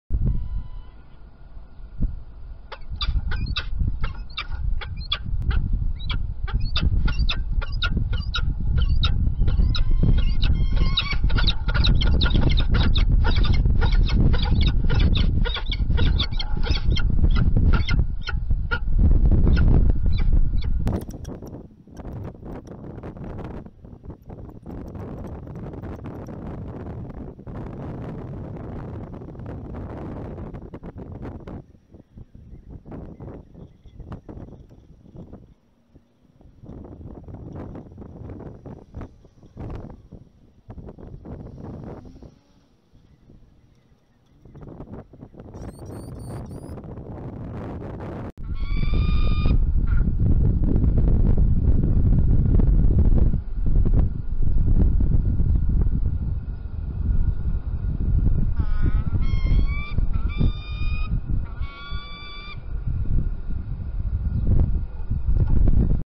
Illkirch Graffenstaden Francja sokół wędrowny-Val i Lucky po sezonie lęgowym-2021 05 31
Nagrane z kanału Faucons Pelerins Illkirch